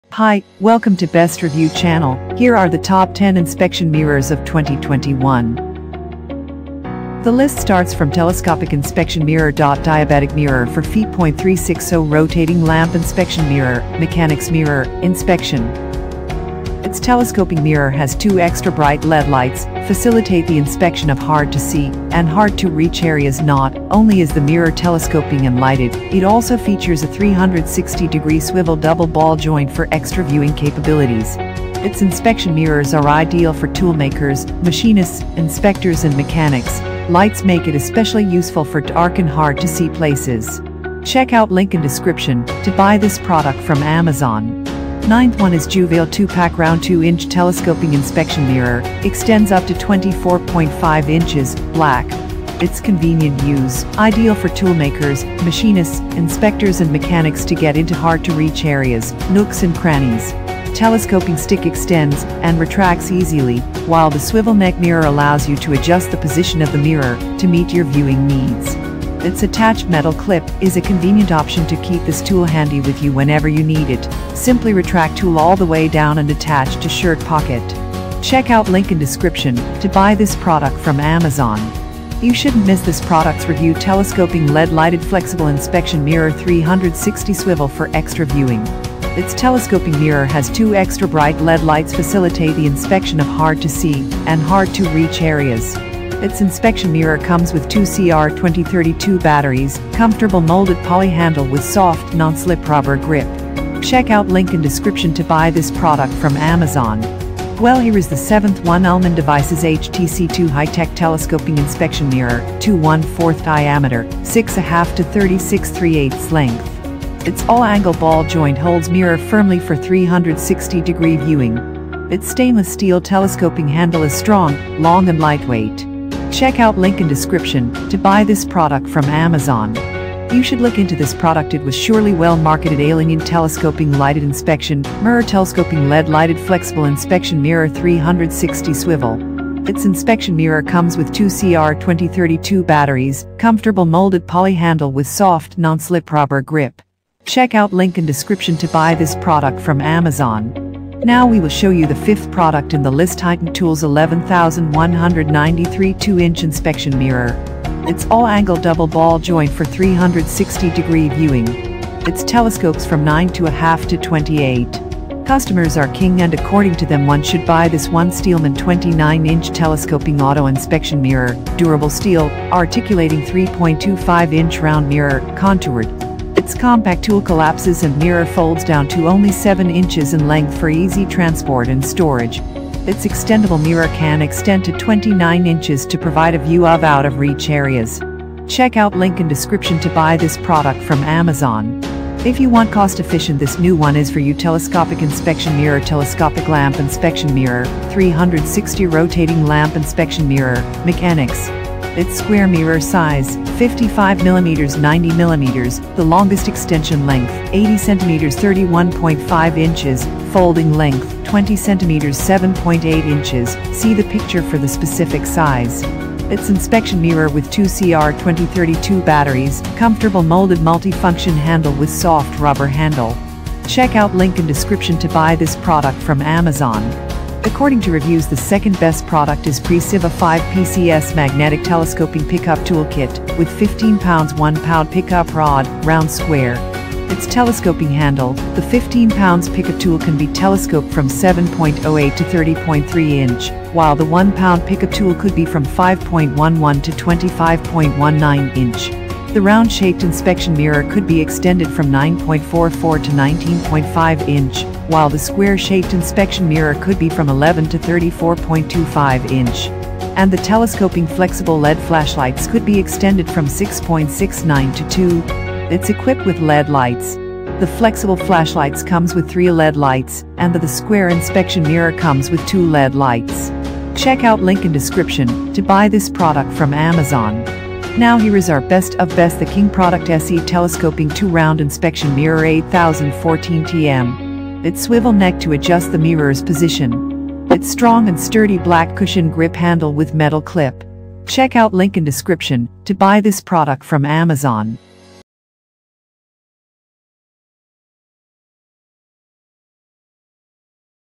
👉 Top 10 Inspection Mirrors 2021 (Review Guide)
👉 1
SE Telescoping 2" Round Inspection Mirror - 8014TM
👉 2
Preciva 5PCS Magnetic Telescoping Pick-up Tool Kit with 15lb/1lb Pick Up Rod, Round/Square
👉 3
Telescopic Inspection Mirror ，Telescopic lamp Inspection Mirror, 360 Rotating lamp Inspection Mirror, Mechanics
👉 4
Steelman 29-Inch Telescoping Auto Inspection Mirror, Durable Steel, Articulating 3.25-Inch Round Mirror, Contoured
👉 5
Titan Tools 11193 2 Inch Inspection Mirror
👉 6
Ailenyun Telescoping Lighted Inspection Mirror，Telescoping LED Lighted Flexible Inspection Mirror 360 Swivel
👉 7
Ullman Devices HTC-2 High-Tech Telescoping Inspection Mirror, 2-1/4" Diameter, 6-1/2" to 36-3/8" Length
👉 8
Telescoping LED Lighted Flexible Inspection Mirror 360 Swivel for Extra Viewing
👉 9
Juvale 2-Pack Round 2-Inch Telescoping Inspection Mirror, Extends up to 24.5 Inches, Black
👉 10
Telescopic Inspection Mirror.Diabetic mirror for feet.360 Rotating lamp Inspection Mirror, Mechanics Mirror, Inspection

After 100's of customers and editors reviews of Top 10 Inspection Mirrors, we have finalised the above mentioned Best 10 products.